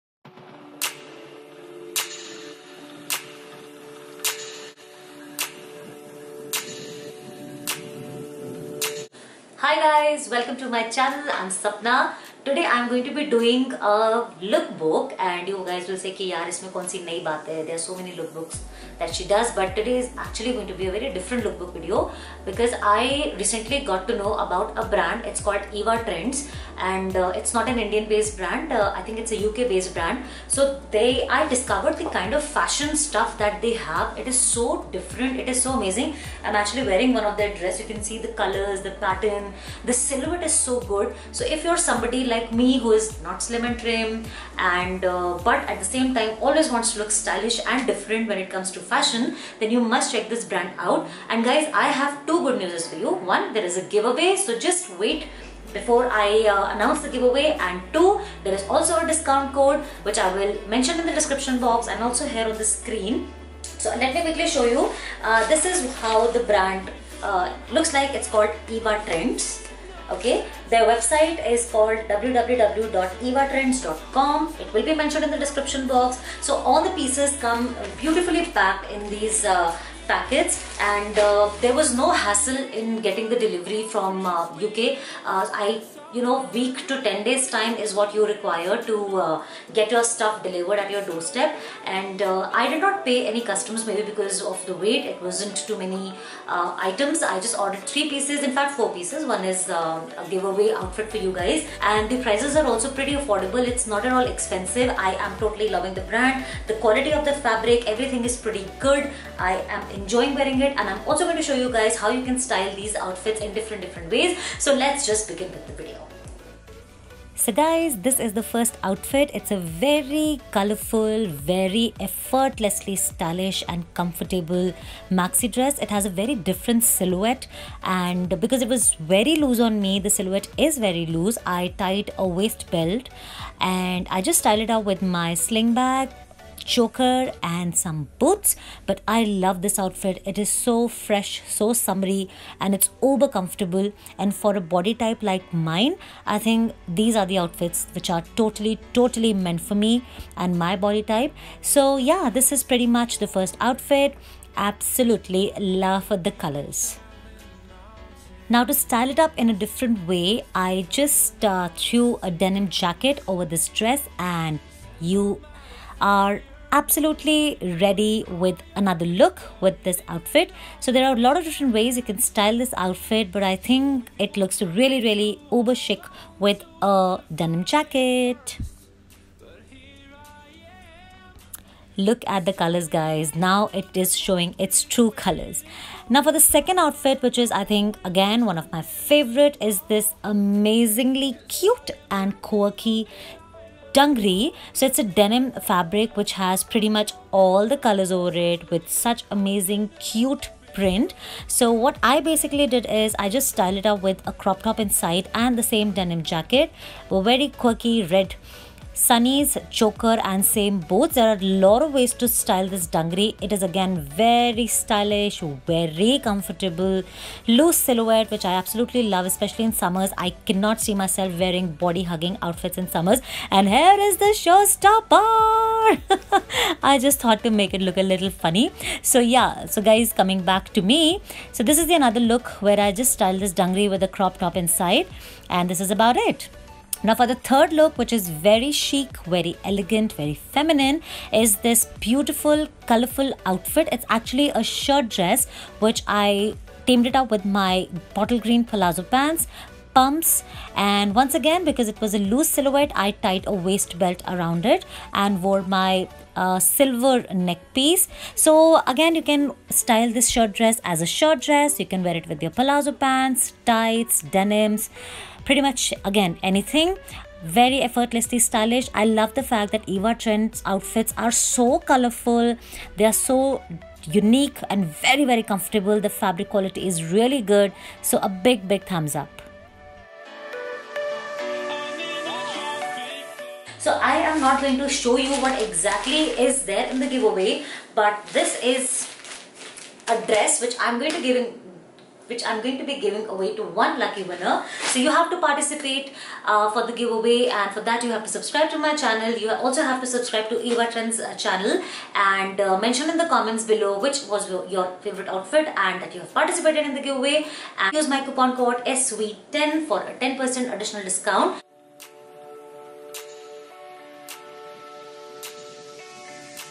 LOOK TRENDY IN SUMMERS WITHOUT SHORT CLOTHES FEAT. EVA TRENDS + GIVEAWAY | shimmerandmusebysapna
Hi angels,
This video is all about effortless, comfortable and stylish summer fashion, without showing much skin.I recently got to know about  a brand, Eva Trends, which is based out of UK. All the pieces in their collection are so edgy, colourful, effortlessly stylish and comfortable.

Below are the links to the website & pieces i got.